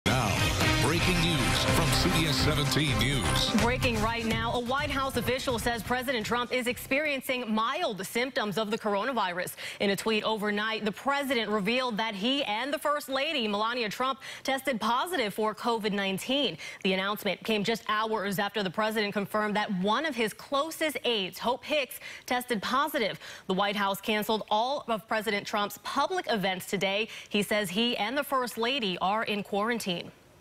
Trump experiencing 'mild' symptoms after testing positive for coronavirus
President Trump and first lady Melania Trump both tested positive for the coronavirus on Thursday night.